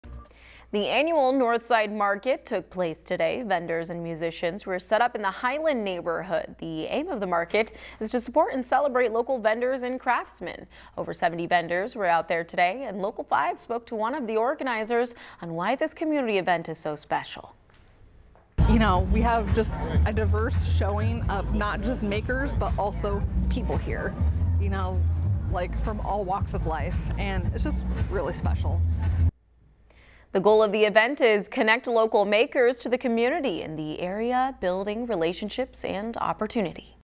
Highland, Oak Park neighborhoods host Northside Market
The goal of the event is to connect local makers to the community, building relationships and opportunities for all.